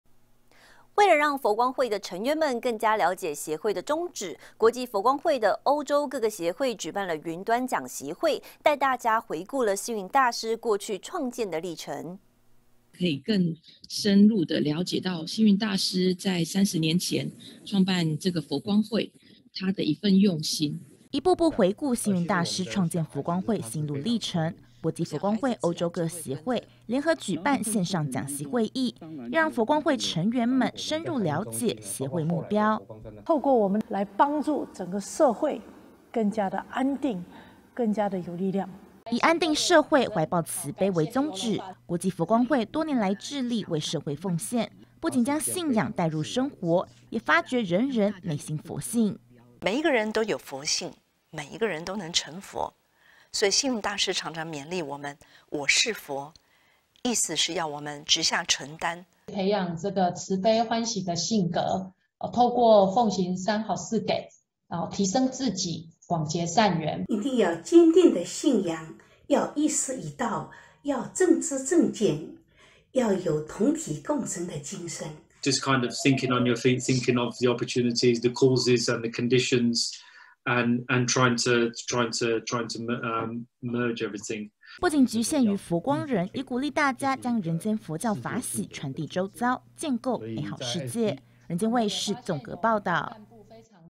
20210402 傳承歡喜 歐洲佛光人分享人間佛教精神
為了讓佛光會的成員們更加瞭解協會宗旨，國際佛光會歐洲各協會舉辦雲端講習會，帶大家回顧星雲大師過往創建歷程，許多成員們也相互分享他們的生活感想，期待把人間佛教的精神傳遞社會